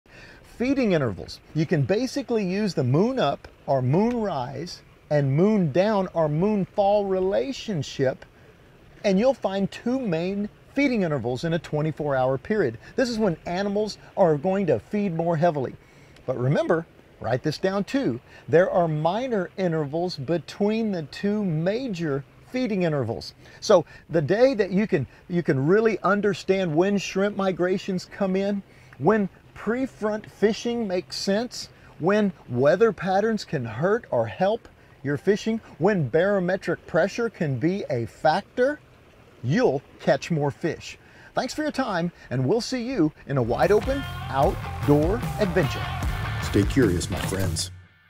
Know When Fish Feed
Feeding intervals in a day. Learn this tip to make your next day on the water even better.

🥇  “We’ll Make You a Better Sportsman”   🦌  🎣